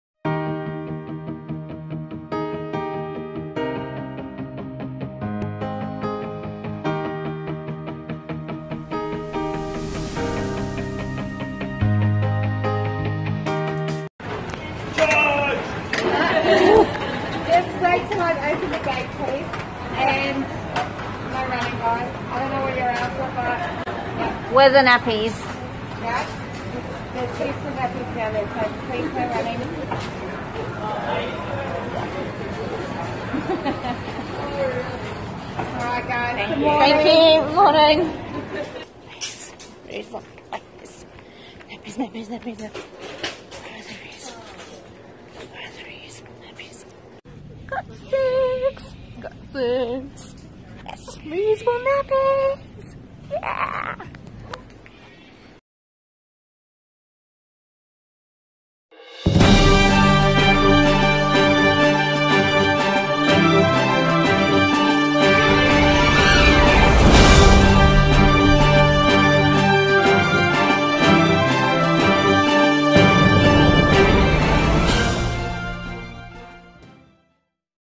Mothers scramble to get Bare and Boho cloth nappies in Aldi special
Eco-warrior mums LINE UP to buy Aldi's designer reusable nappies 'Bare and Boho' for $14.99 - and they sell out in just five minutes
Mothers scramble to get Bare and Boho cloth nappies in Aldi Australia's special buys this week